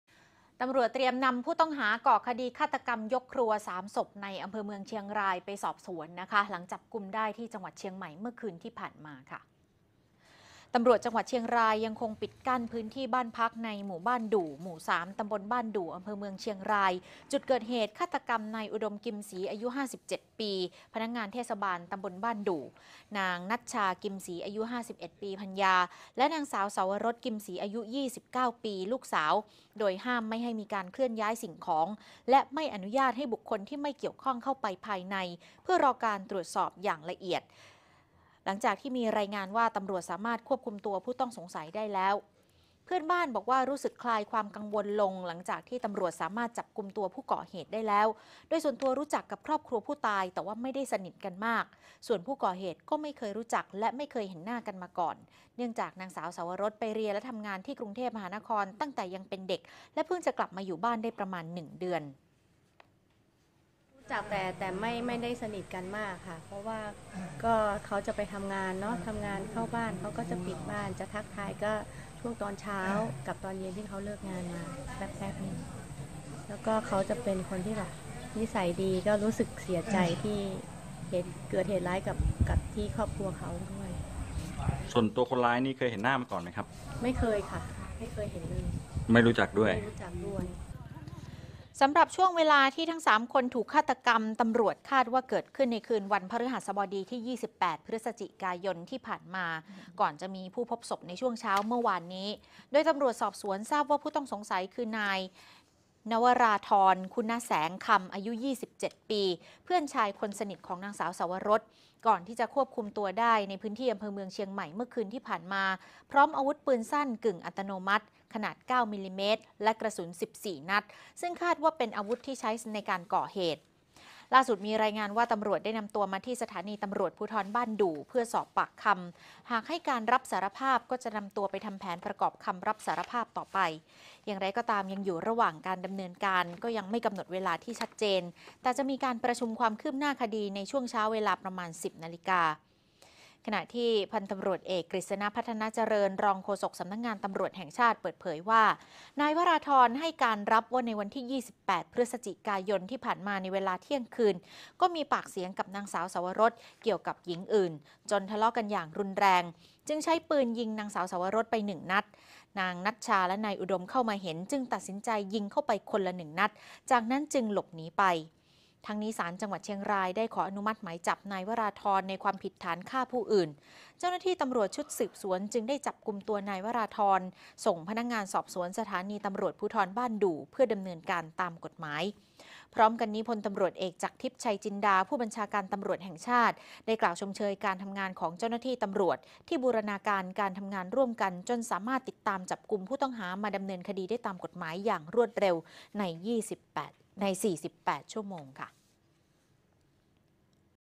เตรียมนำผู้ต้องสงสัยฆ่ายกครัว 3 ศพไปสอบสวนหลังจับได้ที่ จ.เชียงใหม่
ตำรวจเตรียมนำผู้ต้องสงสัย ก่อคดีฆาตกรรม ยกครัว 3 ศพในอำเภอเมืองเชียงรายไปสอบสวน หลังจับกุมได้ที่จังหวัดเชียงใหม่เมื่อคืนที่ผ่านมา